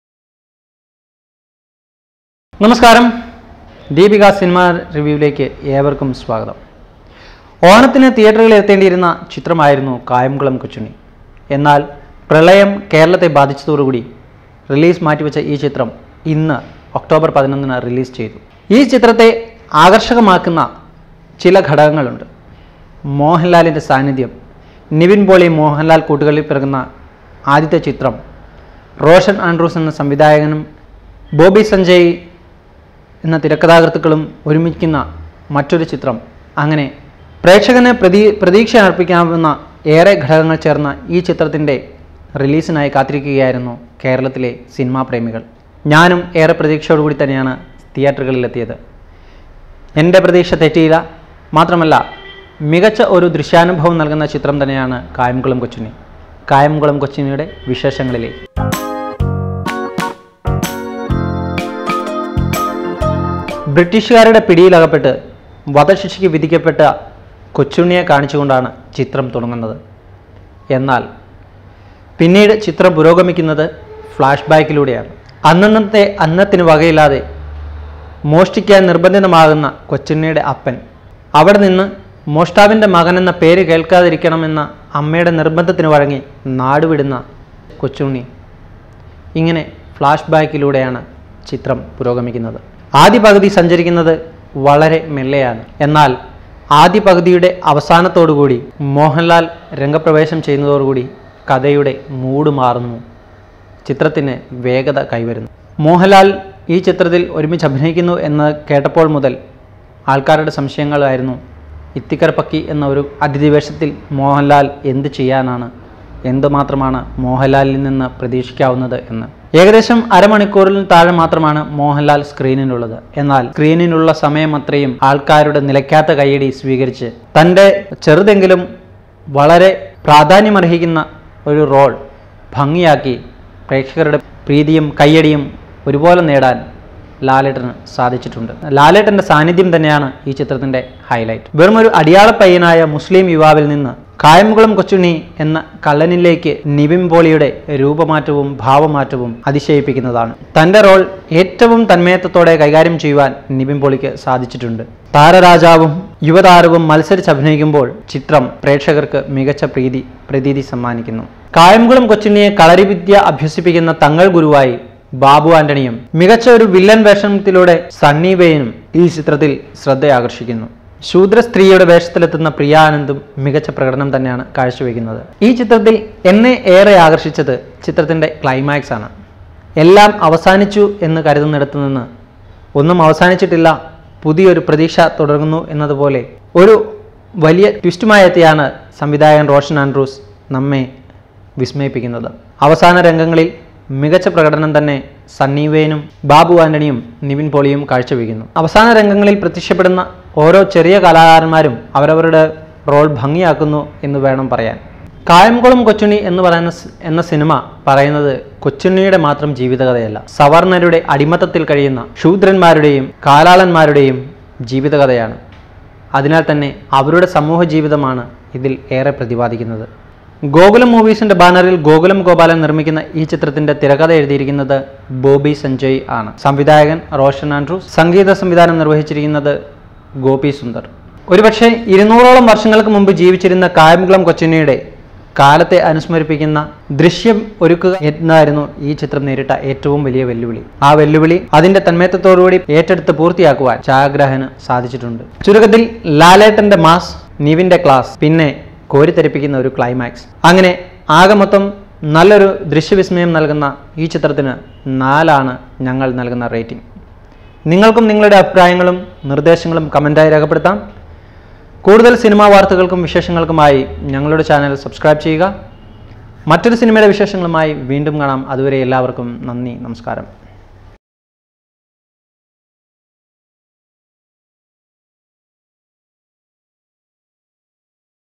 Kayamkulam Kochunni Malayalam Movie Review | Nivin Pauly, Mohanlal
How good is Kayamkulam Kochunni, the latest Malayalam movie starring Nivin Pauly, Mohanlal, Babu Antony, Priya Anand in lead roles.